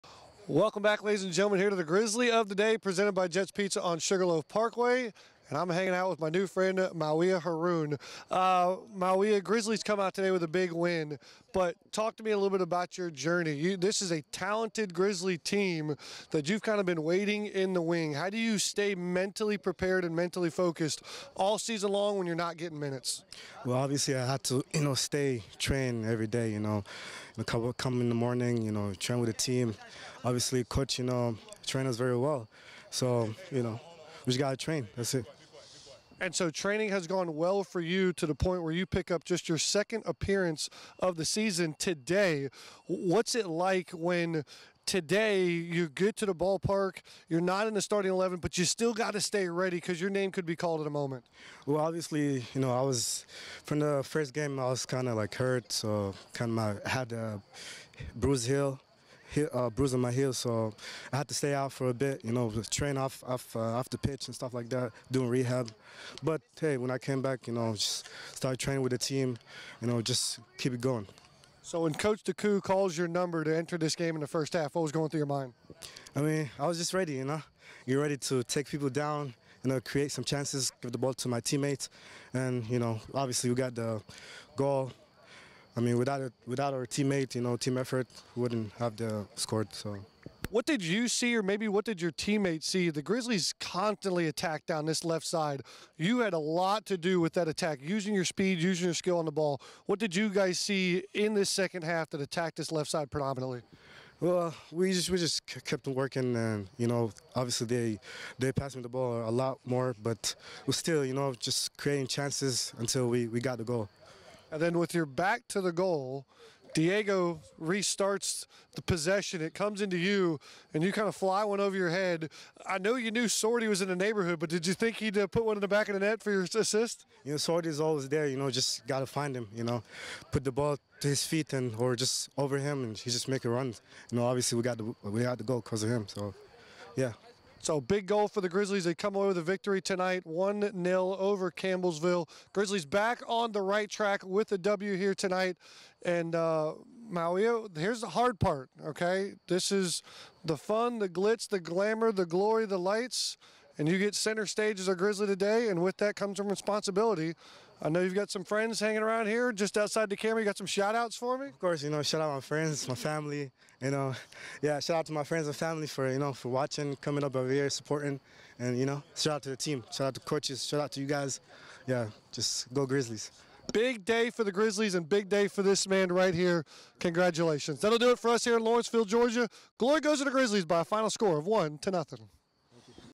Grizzly of the Day: Mawia Haroun vs Campbellsville (Ky.)
Presented by Jet's Pizza on Sugarloaf Parkway, post match highlights and comments from the Grizzlies victory over the Tigers